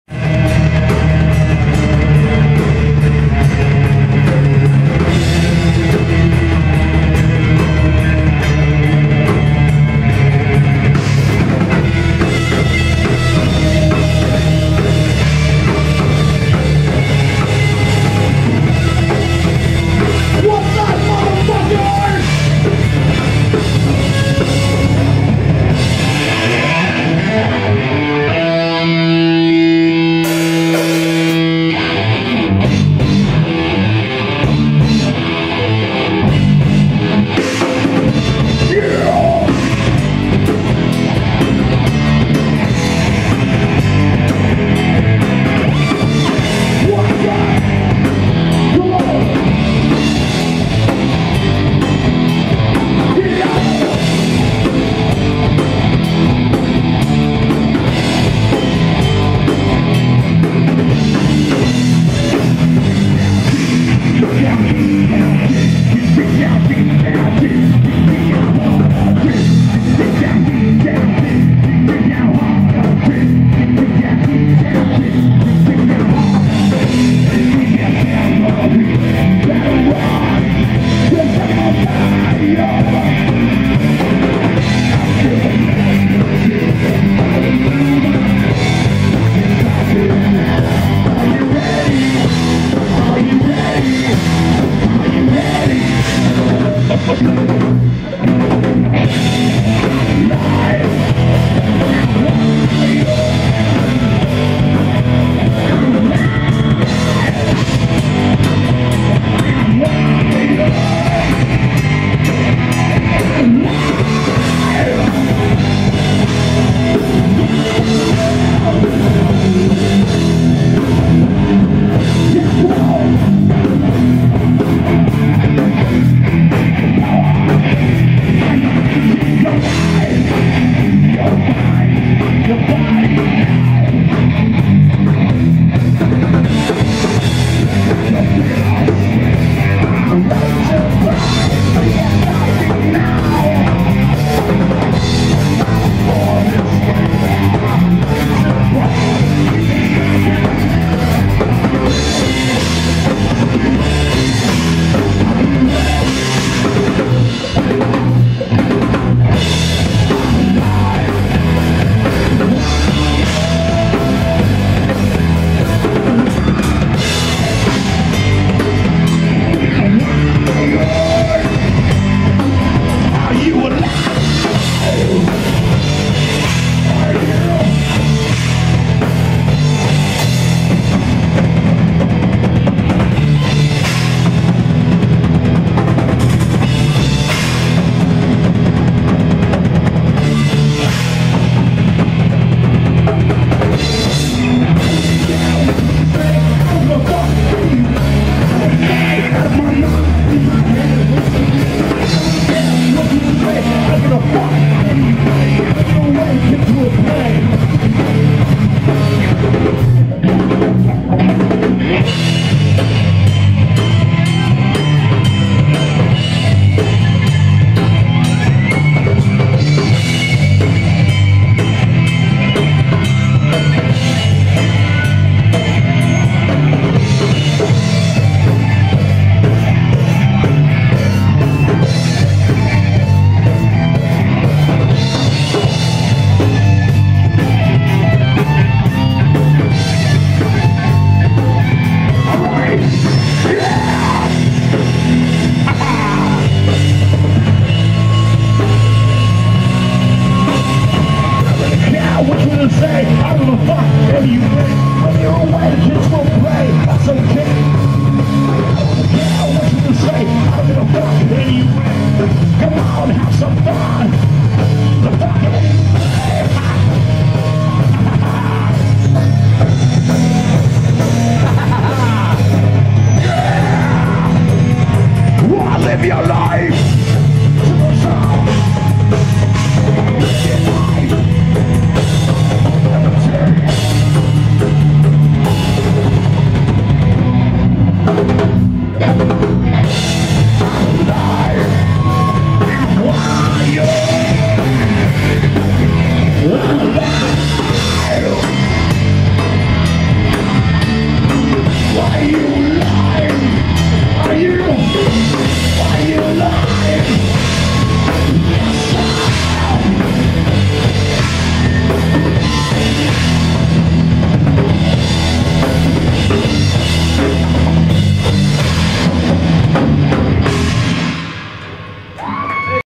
Driven By Turmoil, LIVE!
Bushwackers Bar & Grille with A kick ass Patio great location and decent music venue is located off of Broadway it's just north of Evans